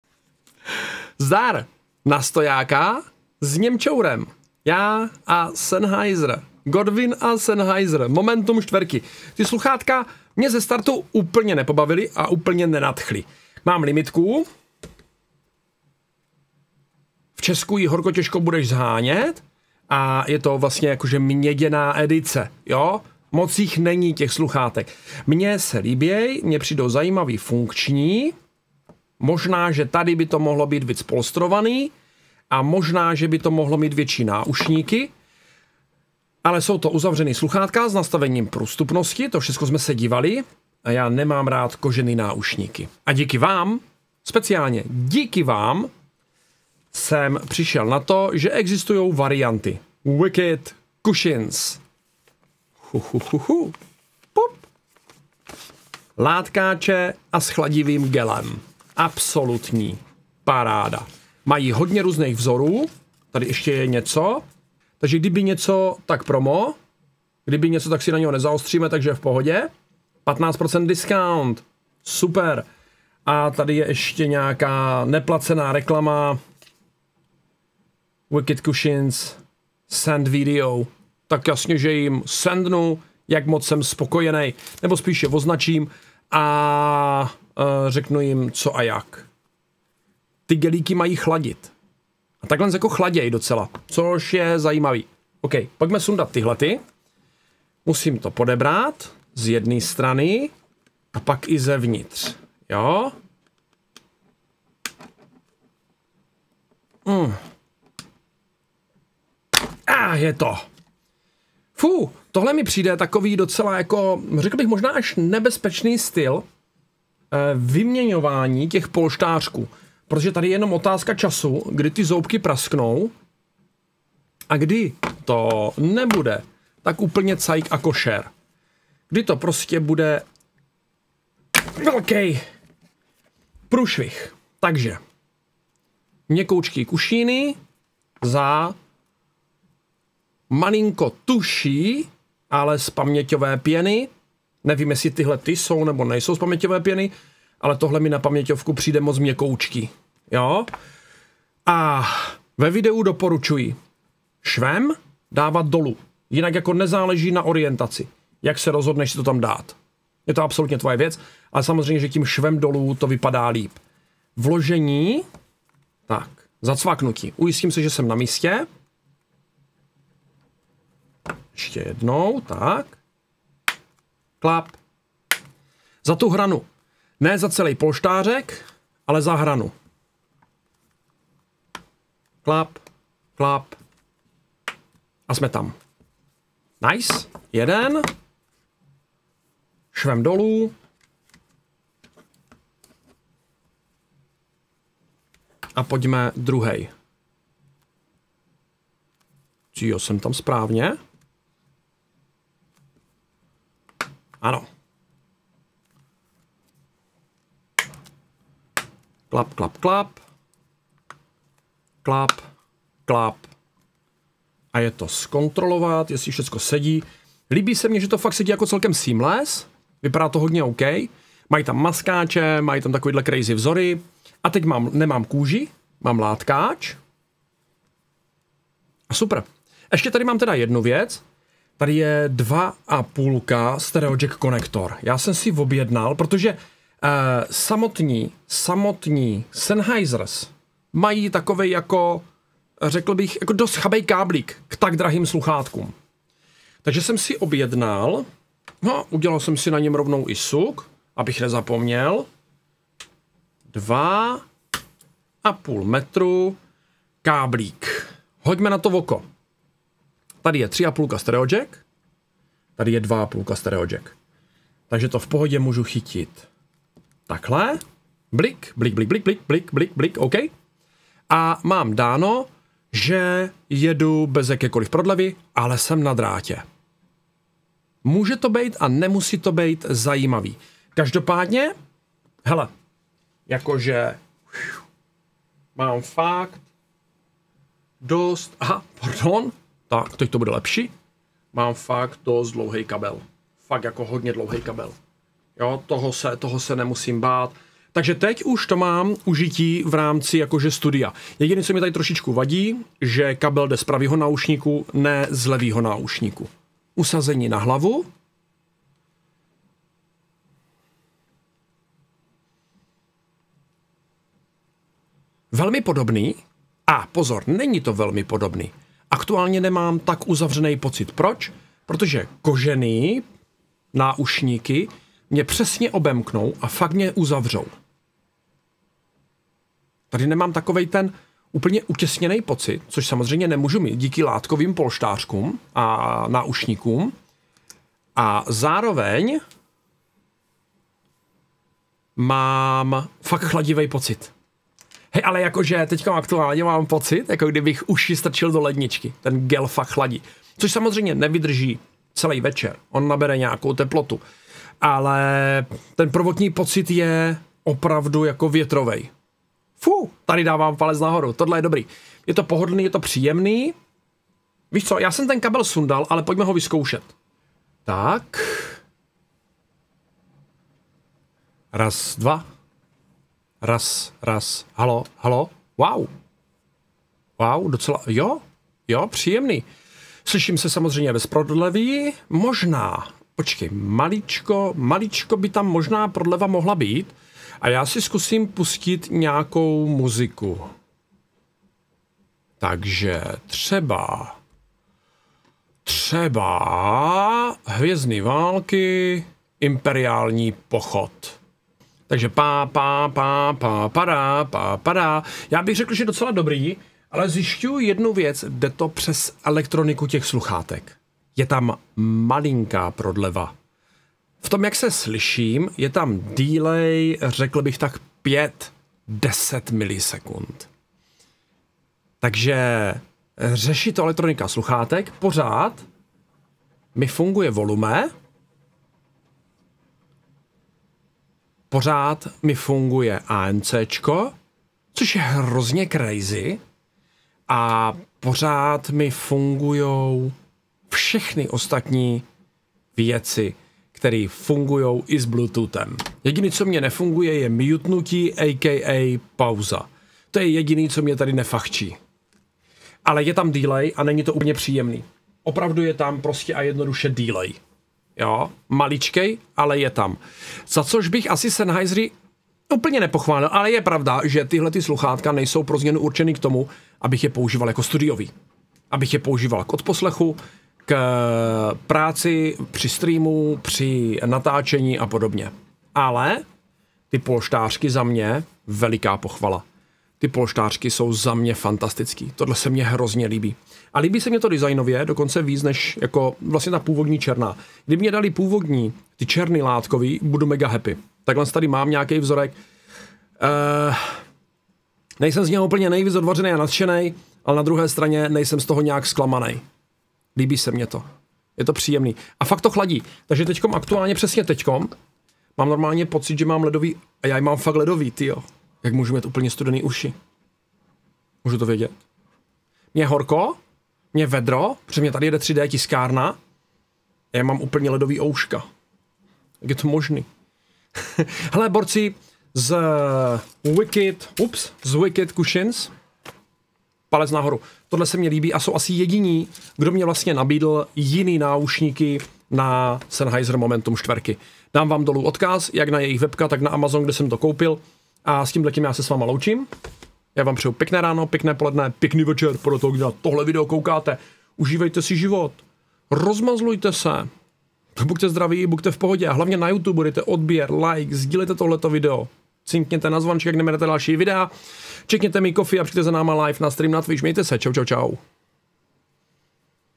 LEPŠÍ NEŽ VÝBORNÉ!!! Aneb pohodlnější náušníky pro Momentum 4
Já fakt nesnáším kožené a koženkové náušníky, takže zde je alternativa - video není sponzorované, produkt jsem si zakoupil na Amazon de